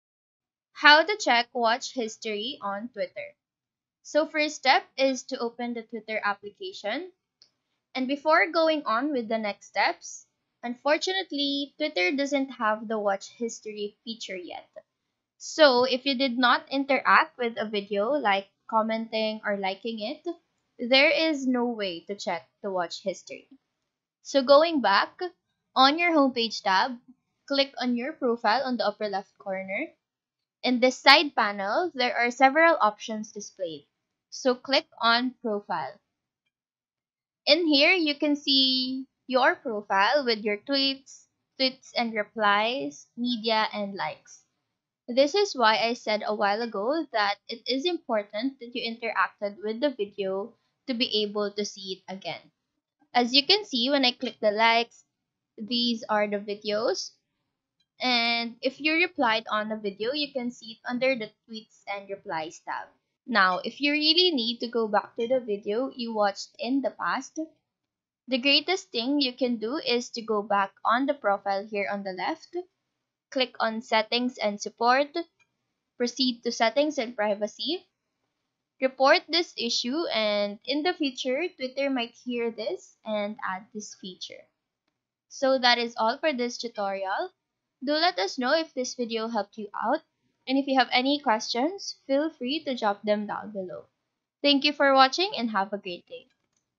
How to Check Watch History on Twitter (2023)
Learn‎‎‏‏‎ ‎How to Check Watch History on Twitter

In this video I will show you‏‏‎ ‎how to check watch history on twitter